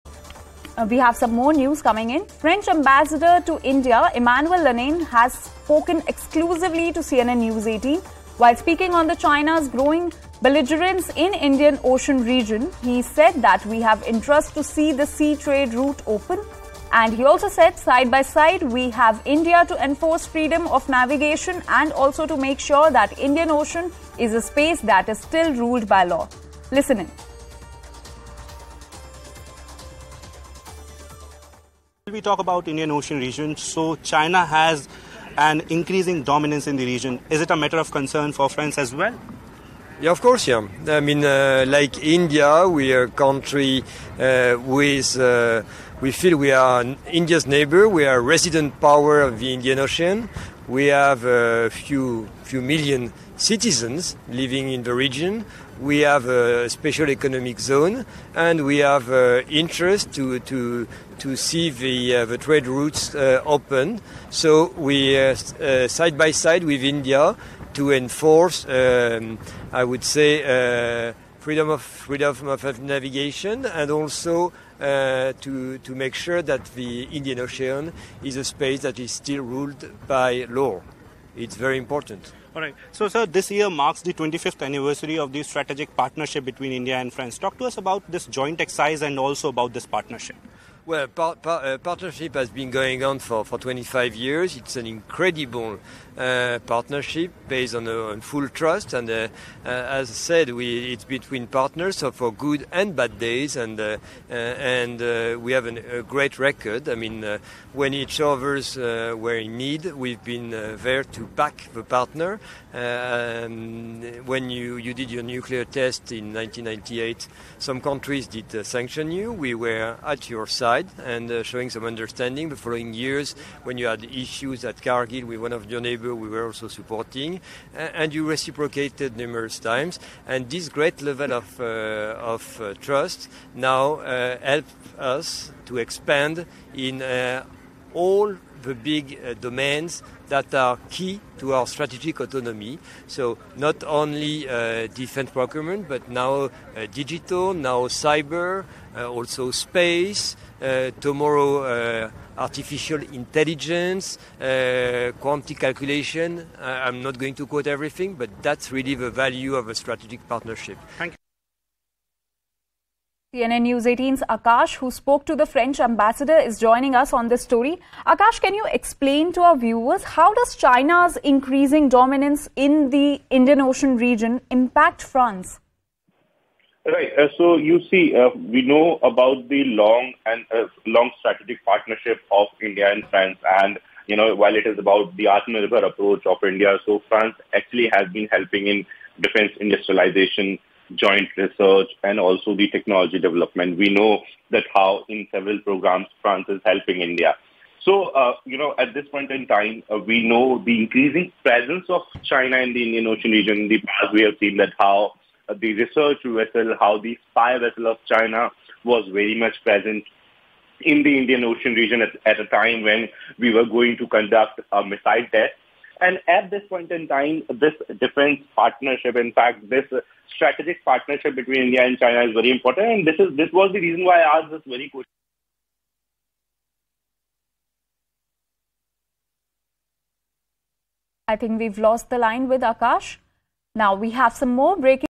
French Ambassador To India On China’s Increasing Presence And Dominance In IOR | Exclusive | News18
China’s increasing presence and dominance in Indian ocean region, it is a matter of concern for France too? Listen in to what French Ambassador to India Emmanuel Lenain has to say in this exclusive conversation with Akash Sharma.

n18oc_exclusive